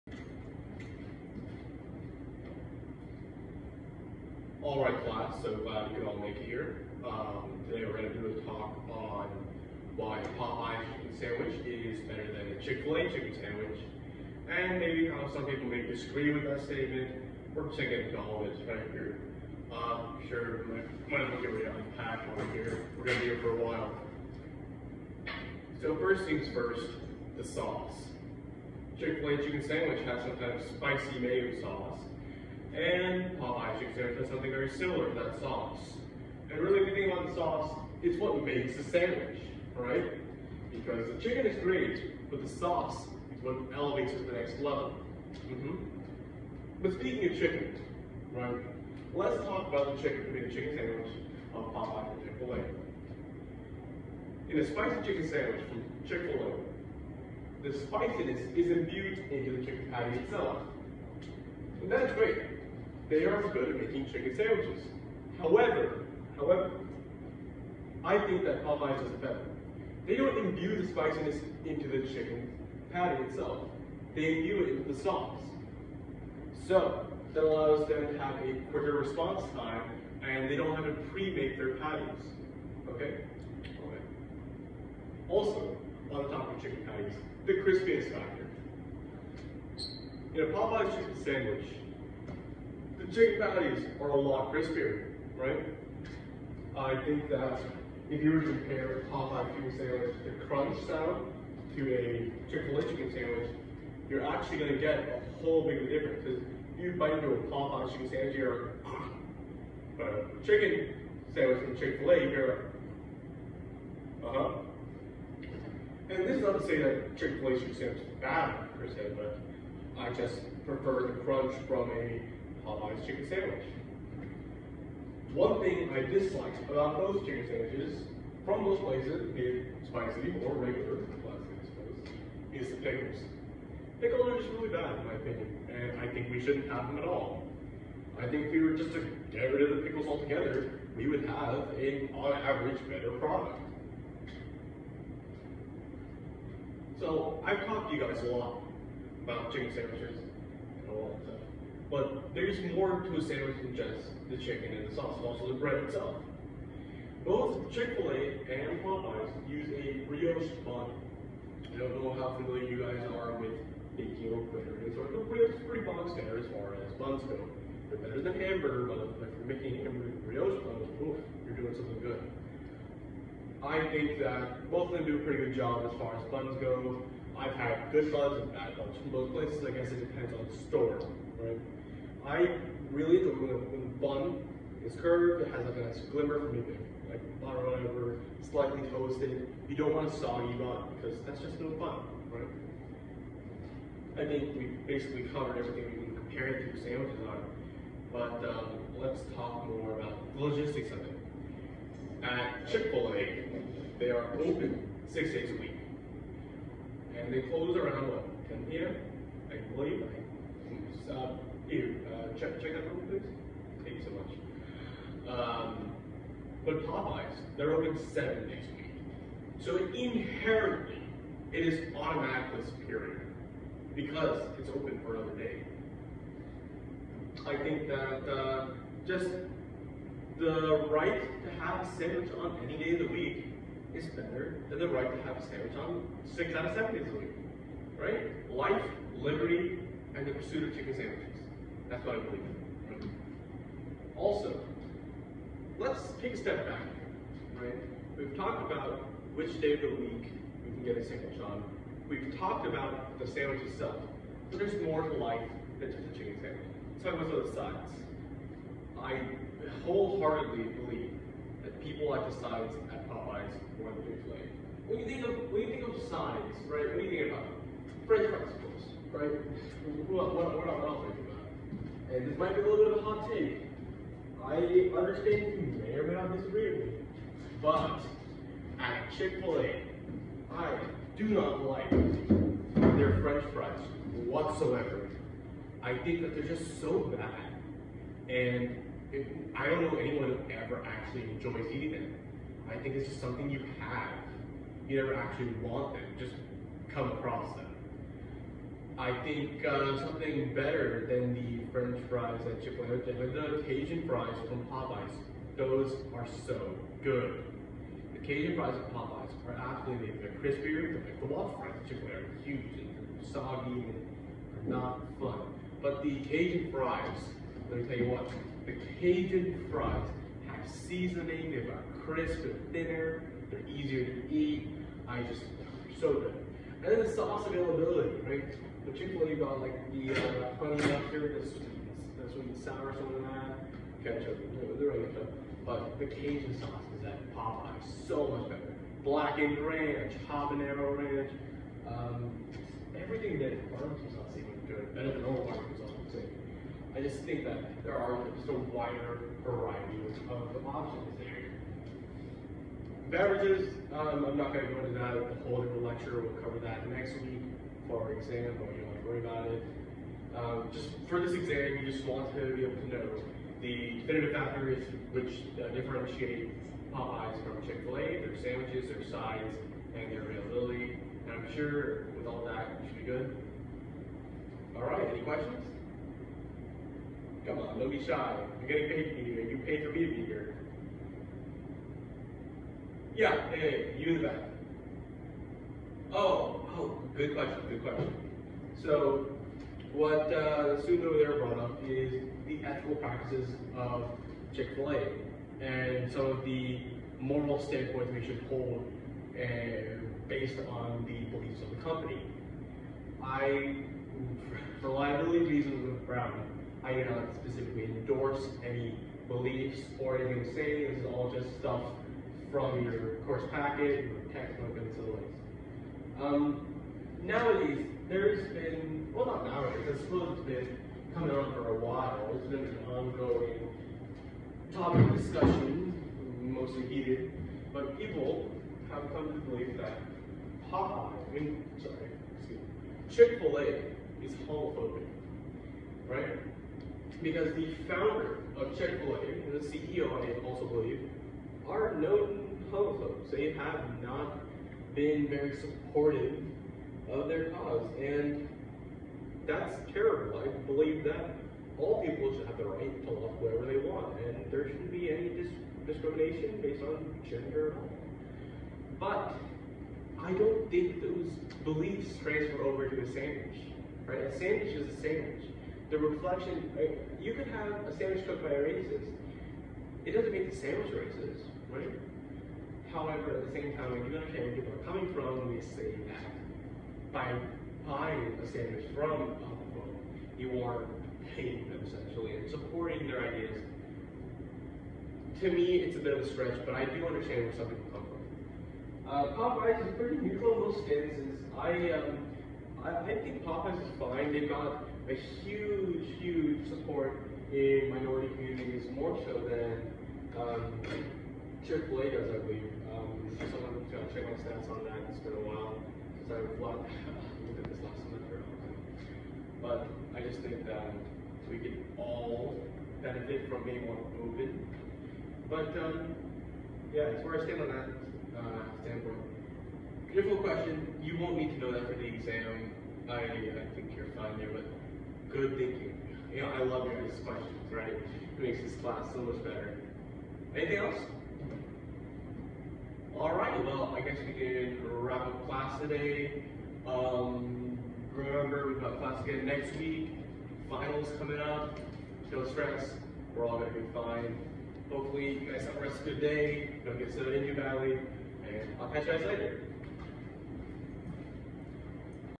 CHK 0100 - Lecture: Chicken Sandwich Talk
Part 1 of a multi-part course (CHK 0100) on chicken food items.
A preliminary lecture regarding Popeyes and Chick-fil-A’s chicken sandwiches. This is meant to serve as a prerequisite to the actual food review coming soon.
NOTE: no sandwiches consumed during this lecture